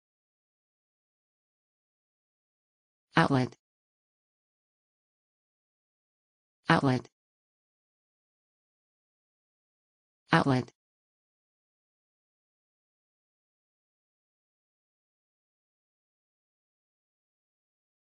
How to say outlet in English
A short video showing how to pronounce outlet.